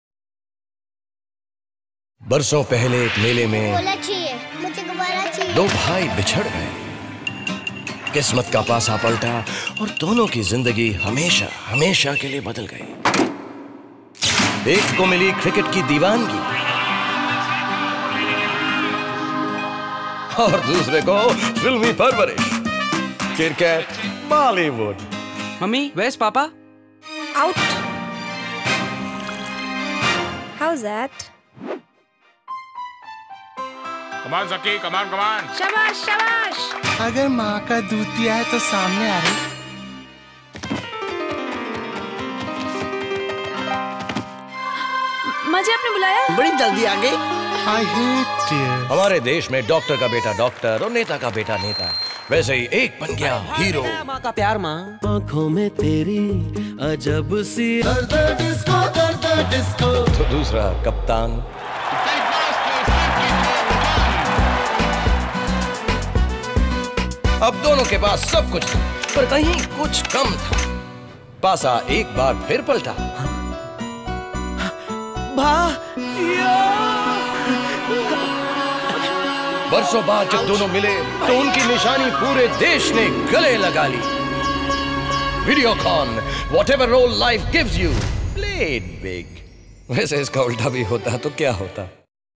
Videcon
A spot on a possible role reversal between Shahrukh & Dhoni.

CLIENT: VIDEOCON
AGENCY: O&M MUMBAI
CREATIVE: SHUMO, HEERAL & SUKESH
DIRECTOR: PUSHPENDRA MISRA
PRODUCTION HOUSE: FLYING SAUCER